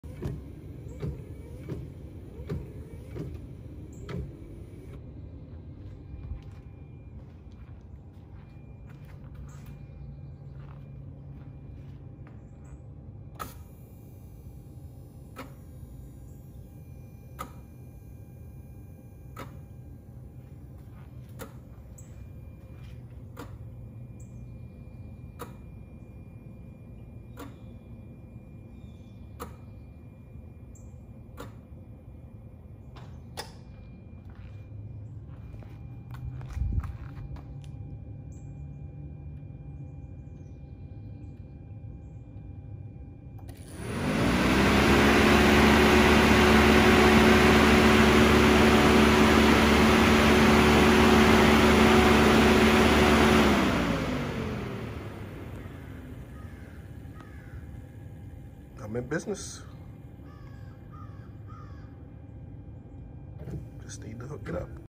Nissan Titan condenser fan test
Testing the unused fan plug with a new add-on condenser fan.